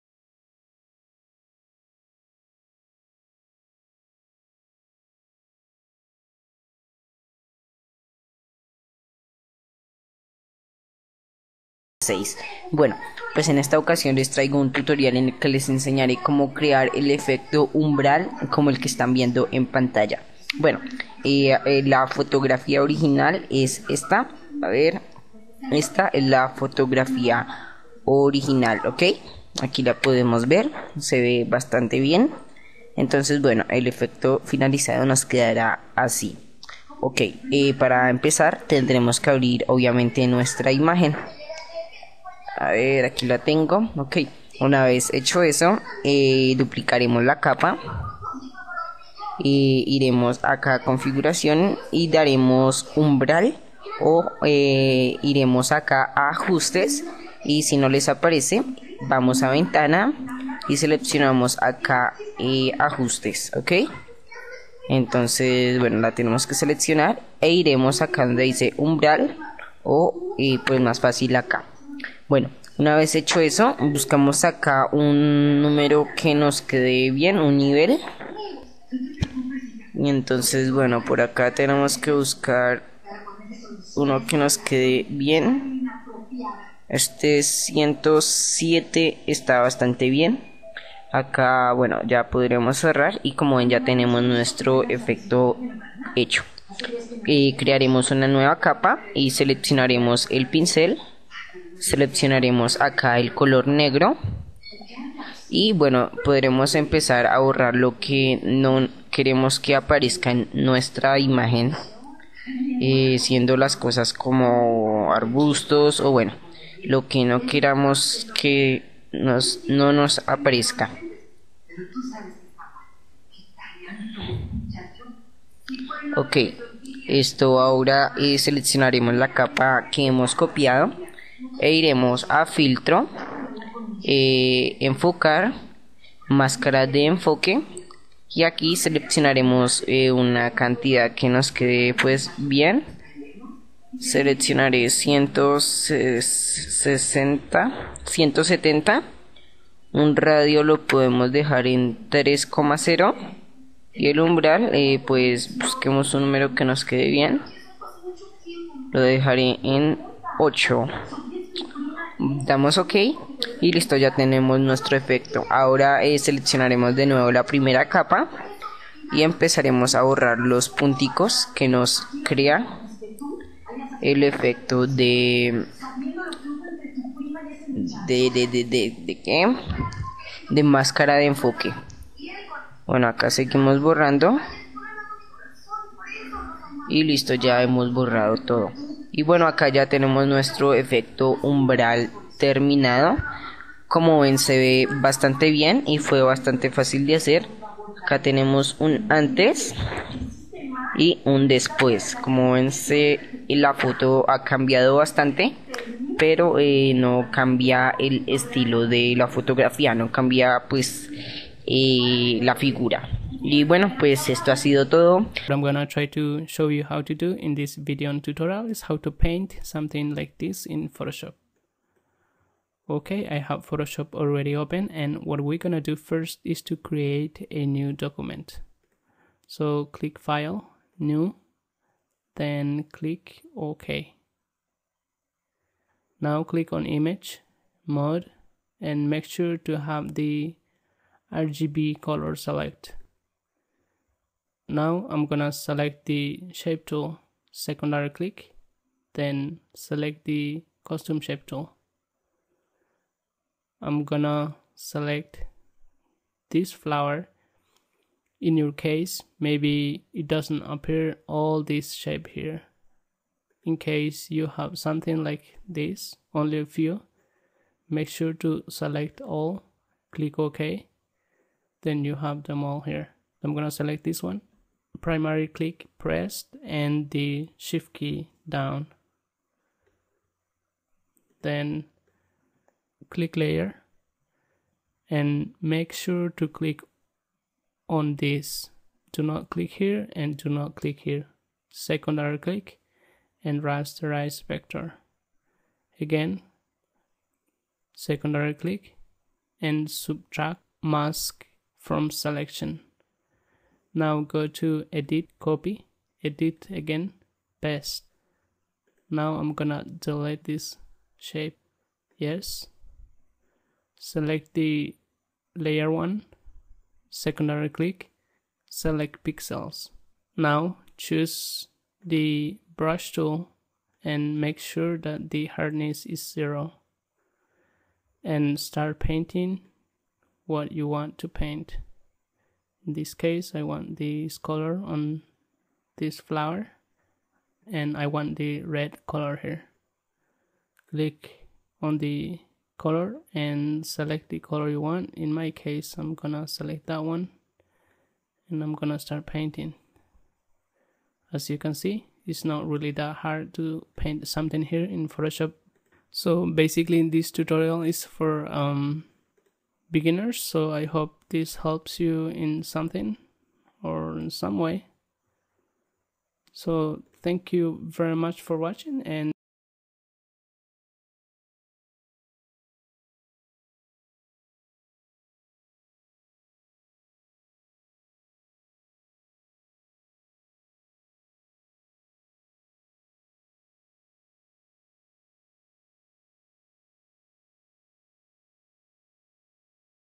Efecto Face Silhoette Umbral Photoshop CS6_Tutorial 2016
Te enseño como se hace el popular efecto de Face Silhoette Umbral de manera fácil.
 En este tutorial de photoshop cs6 2016.